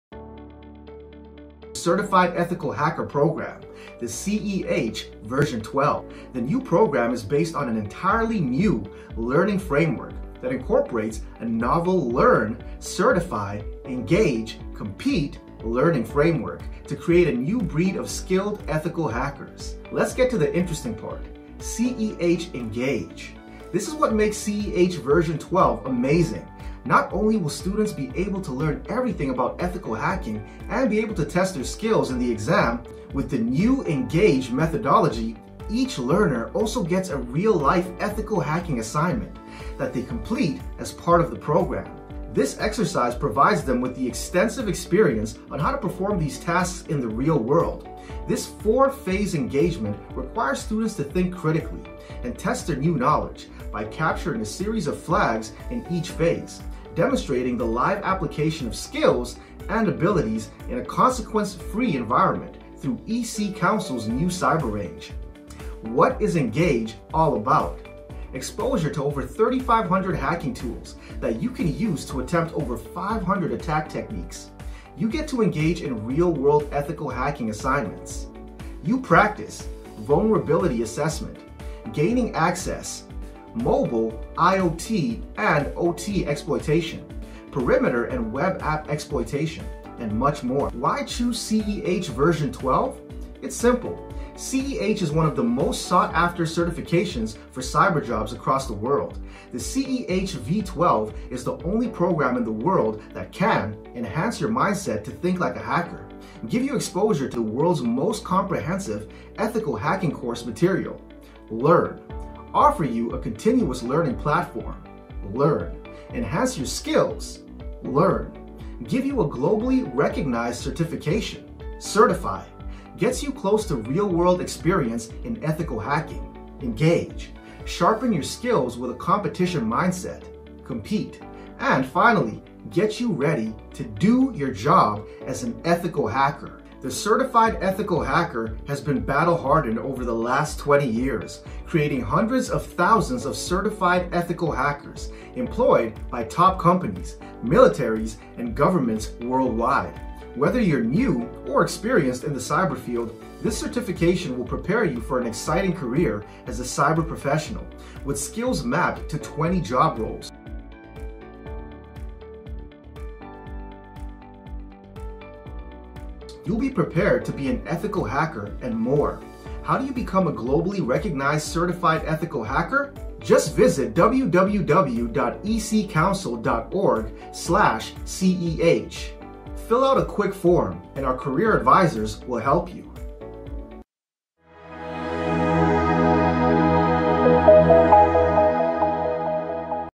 A Guide to C|EHv12 Engage - Phase 3 Learning Framework: Engage
In its 12th version, the Certified Ethical Hacker provides comprehensive training, hands-on learning labs, practice cyber ranges for engagement, certification assessments, cyber competitions, and opportunities for continuous learning into one comprehensive program curated through our new learning framework: 1. Learn 2. Certify 3. Engage 4. Compete.
CEH Engage:

The C|EH v12 program helps you develop real-world experience in ethical hacking through the hands-on C|EH practice environment. The C|EH Engage equips you with the skills to prove that you have what it takes to be a great ethical hacker.

New to C|EH v12, students will embark on their first emulated ethical hacking engagement. This 4-phase engagement requires students to think critically and test the knowledge and skills gained by capturing a series of flags in each phase, demonstrating the live application of skills and abilities in a consequence-free environment through EC-Council’s new Cyber Range.

As you complete your training and hands-on labs, the C|EH Engage lets you apply everything you have learned in a mock ethical hacking engagement. This 4-part security engagement gives you a real ethical hacking engagement experience from start to finish against an emulated organization. Using our capture-the-flag-style range, you will complete your engagement by answering “flag” questions as you progress.
Your Mission
Whether this is your first engagement or you’re honing your skills, get ready to test your ethical hacking knowledge like never before! Once you’ve practiced through the hands-on guided labs, it’s time to apply your knowledge, take on the hacker persona, and find the vulnerabilities and weaknesses in ABCDorg—all built in our C|EH Engage (practice range).

Target Organization Characteristics
• ABCDorg is a nationwide IT/ITES organization
• Realistic segmented networks
• Demilitarized zones (DMZs) and private subnets stretch across the infrastructure to support various business units
• Various application servers and services support ABCDORG Operations
• Real networks, real operating systems, and real applications
• Private, dedicated access—no shared resources
• Fully automated network deployment with EC-Council’s Cyber Range
• 24x7 browser-based access

Objectives

Armed with your attack platform (Parrot OS) and a plethora of tools used by ethical hackers, you will embark on a 4-part engagement to assess ABCDORG’s security posture. Follow the process, practice your TTP, and experience the real thing in a controlled environment with no consequences. It’s the ultimate learning experience to support your career as an ethical hacker! Each phase builds on the last as you progress through your ABCDORG engagement.